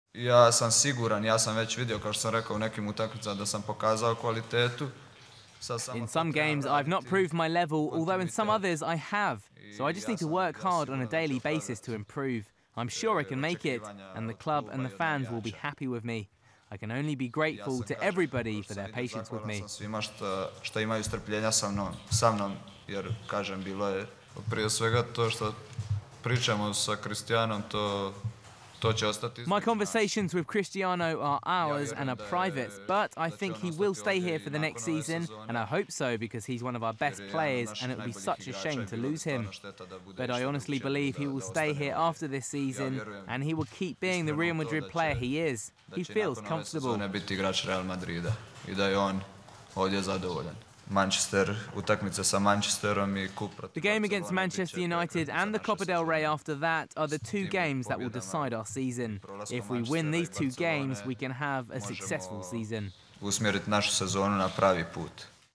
Luka Modric: Cristiano Ronaldo 'comfortable' at Real Madrid
Real Madrid midfielder Luka Modric believes Cristiano Ronaldo will remain at the Santiago Bernabeu next season, and added that their upcoming UEFA Champions League tie with Manchester United and the Copa del Rey semi-final with Barcelona would decide their season.

Luka Modric, Real Madrid midfielder:
"In some games I have not proved my level, although in some others I have. So I just need to work hard on a daily basis to improve. I am sure I can make it, and the club and the fans will be happy with me. I can only be grateful to everybody for their patience with me."

"My conversations with Cristiano (Ronaldo) are ours and are private, but I think he will stay here for the next season and I hope so because he is one of our best players and it would be such a shame to lose him. But I honestly believe he will stay here after this season and he will keep being the Real Madrid player he is. He feels comfortable."

"The game against Manchester (United) and the Copa (del Rey) after that, are the two games that will decide our season. If we win these two games we can have a successful season."